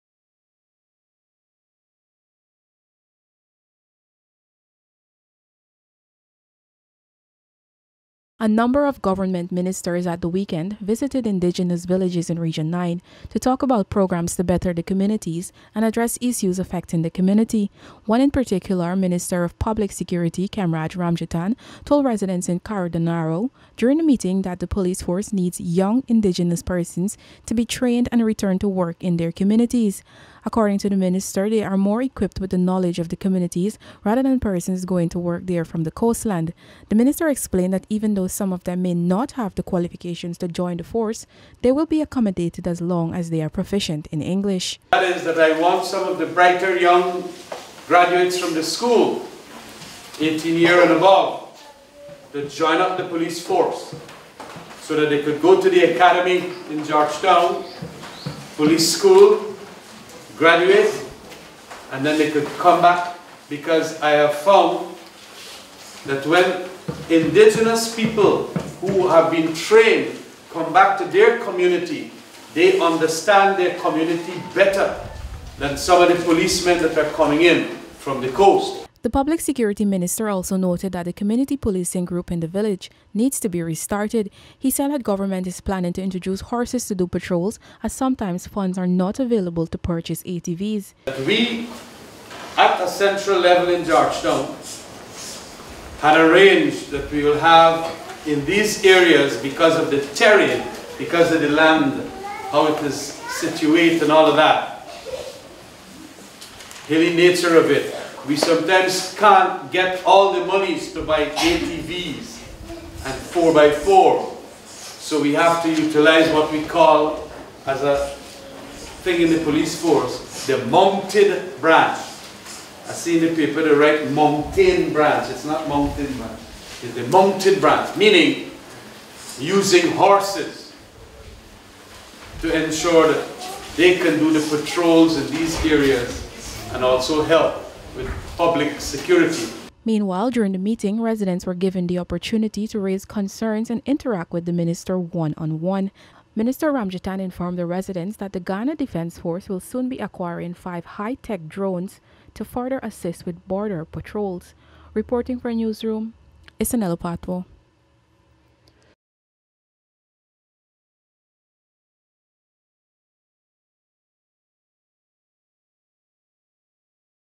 SECURITY MINISTER ENCOURAGES INDIGENOUS YOUTHS TO JOIN POLICE FORCE
Minister of Public Security Khemraj Ramjattan during a Government outreach to Karaudarnau Village in Region 9, encouraged young indigenous persons to become members of the Guyana Police Force and return home to better their safeguard their communities. Isanella Patoir reports…